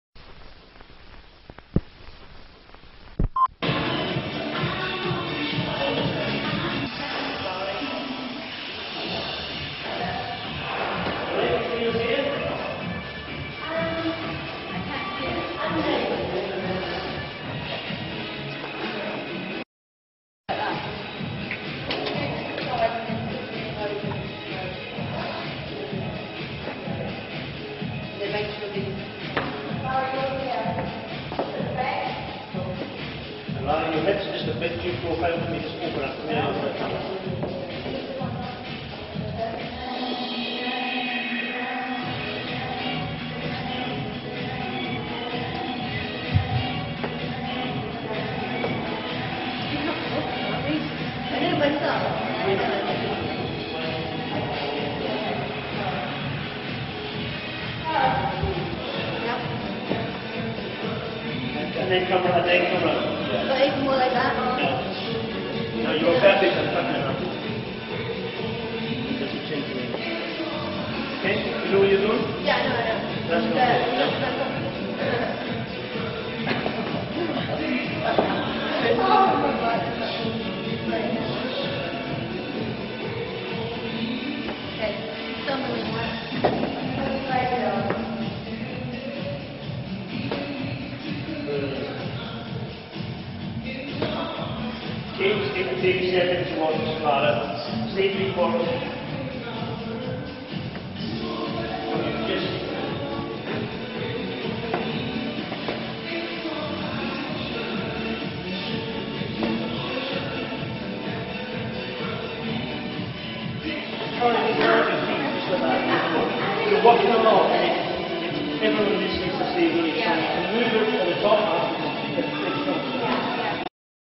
GameTap Lara Croft Models in Latex
To celebrate the release of Tomb Raider: Anniversary, and the 10th anniversary of the series and its heroine, GameTap staged a photo shoot reuniting four former Lara models with then current Lara, Karima Adebibe. The photo shoot took place in October 2006. The GameTap Tomb Raider retrospective was released in May 2007.